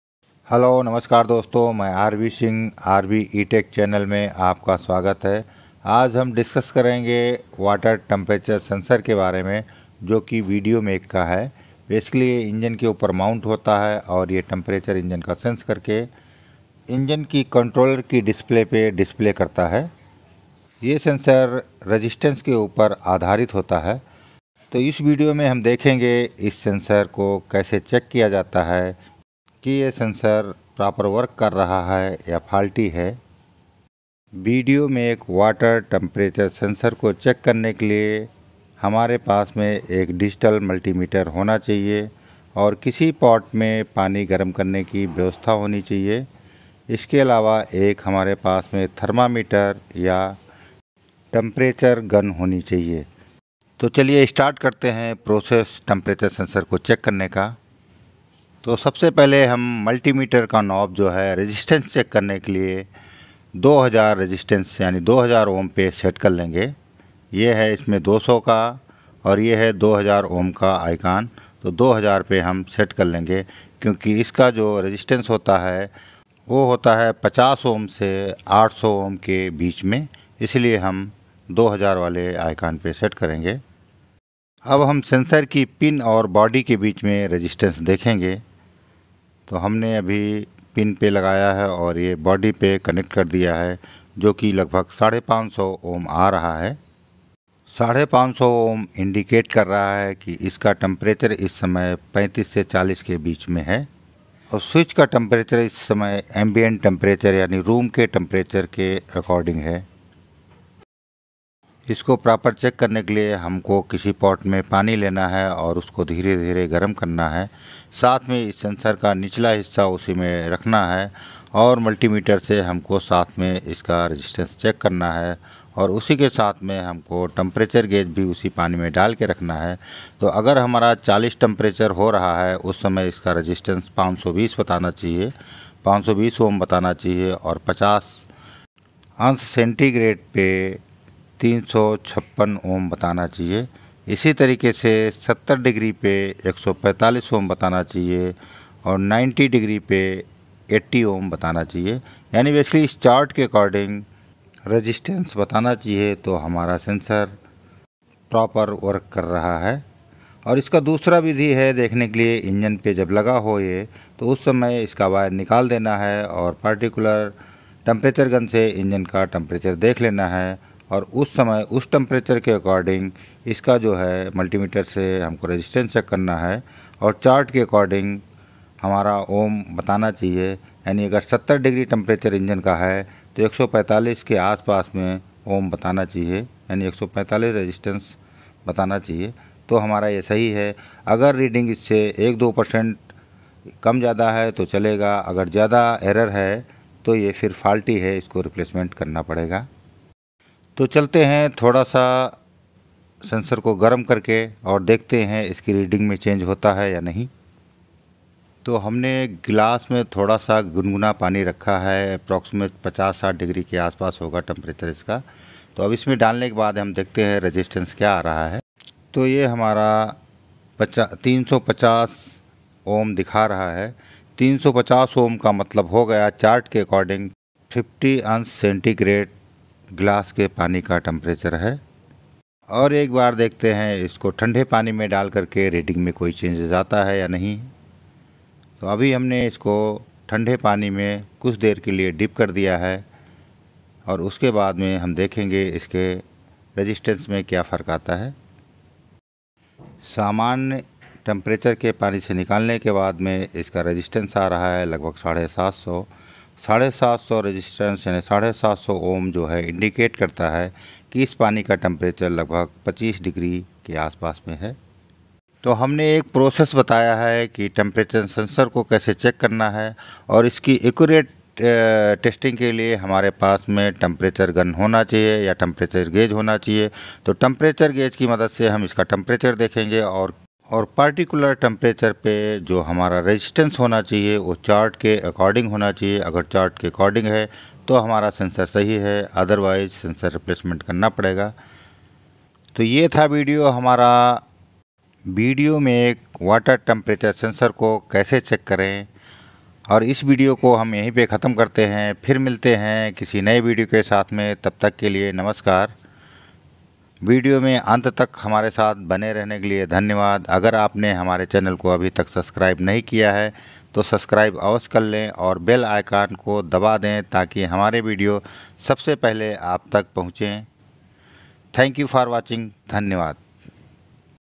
How to check VDO make water temp sensor/Engine Temperature 🌡️ Sensor@rb_etech_youtube_channel
In this video we will watch about VDO make Water temperature Sensor,how to check VDO make water temp Sensor